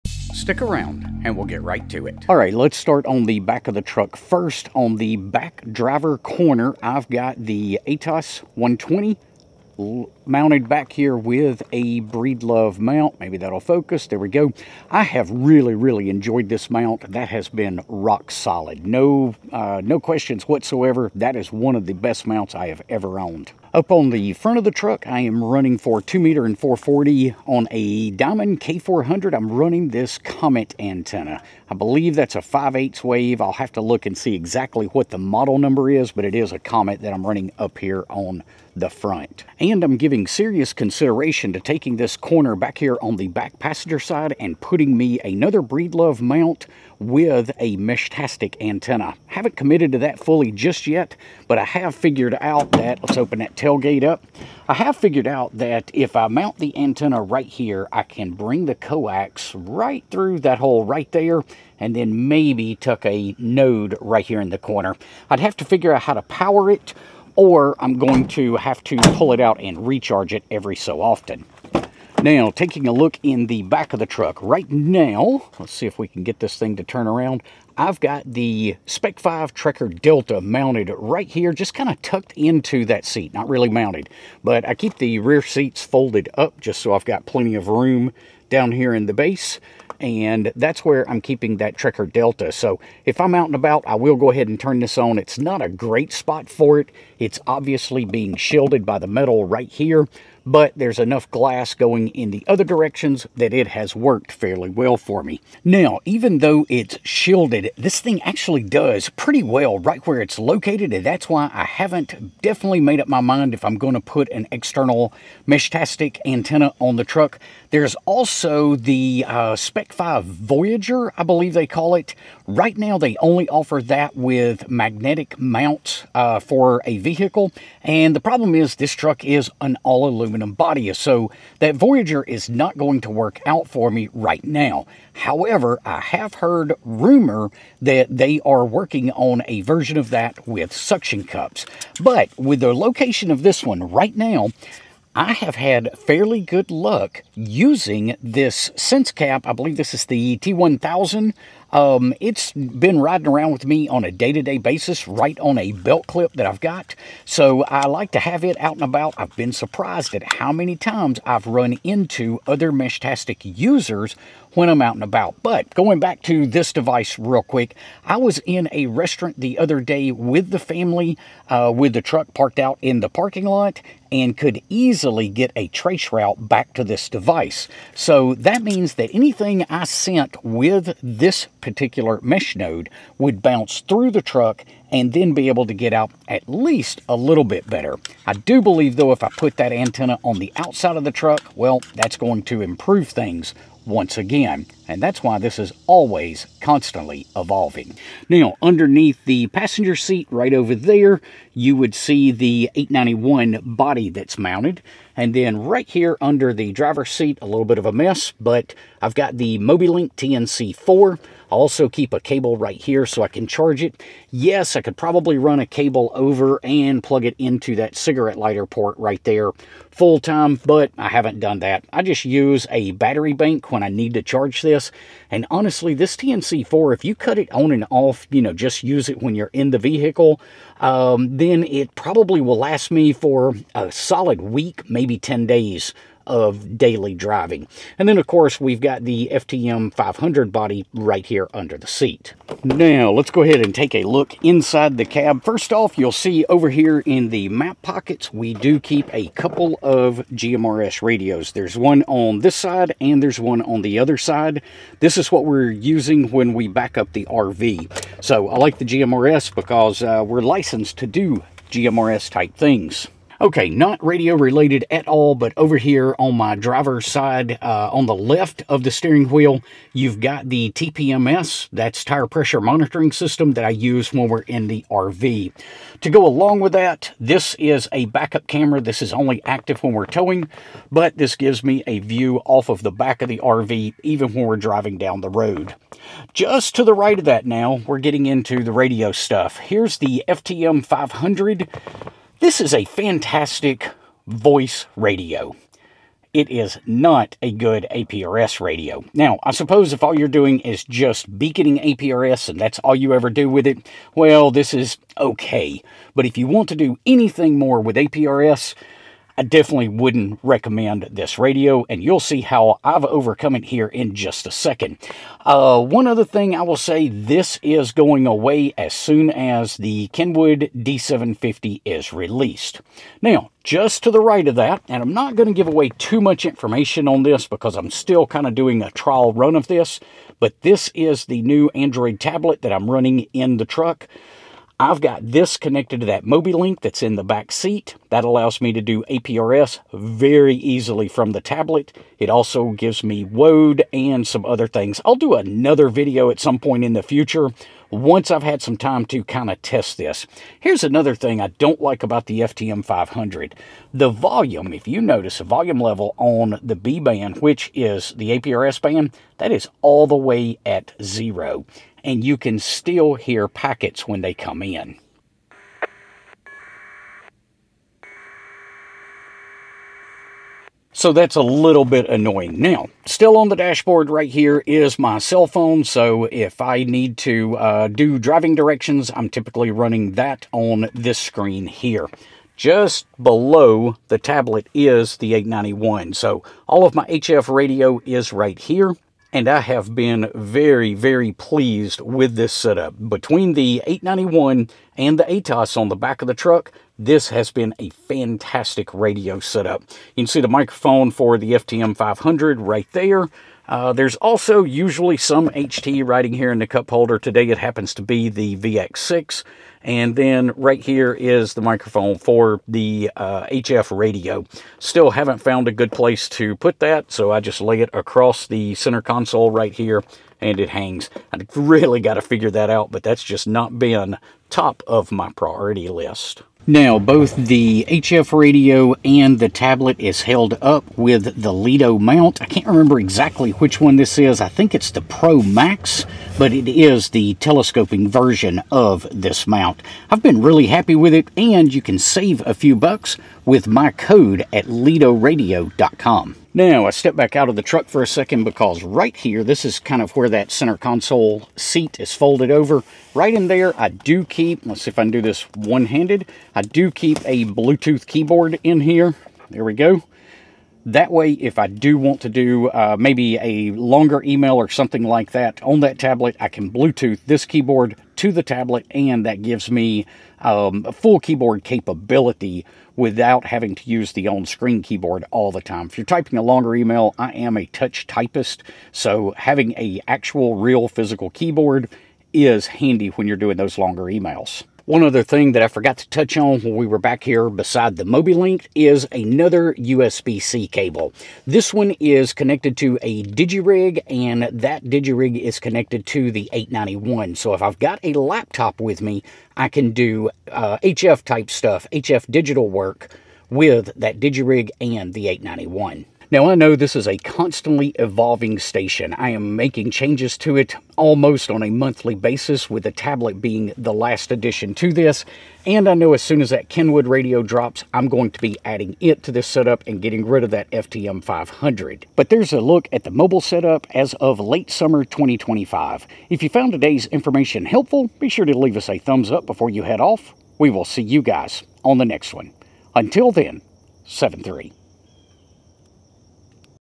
Ultimate Ham Radio Truck Setup Yaesu 891 + FTM 500 + Meshtastic + Winlink in F250
🔧 Ultimate Mobile Ham Radio Setup in a Ford F250 | Yaesu 891, FTM-500, Meshtastic, APRS, & Winlink 📡
Take a tour of my fully-equipped mobile ham radio shack installed in a Ford F250 truck, designed for off-grid communication, emergency preparedness, and DXing on the go! In this video, I walk through my complete mobile setup featuring:
📻 Yaesu FT-891 HF Radio – Reliable 100W portable HF rig for 10m–160m band ops📡 Yaesu FTM-500DR – Dual-band VHF/UHF with APRS and digital fusion capabilities📶 Meshtastic Node – LoRa mesh network integration for off-grid text messaging📨 Winlink RMS Express – Send & receive email over HF using VARA & digital modes
✅ Capabilities include:
 • HF voice communication (SSB)
 • VHF/UHF simplex and repeater ops
 • APRS tracking & messaging
 • Digital modes: Winlink email, VARA HF, JS8Call, FT8
Whether you're into portable ham radio, SHTF communication, or just love building out a communications rig for the road, this setup has you covered.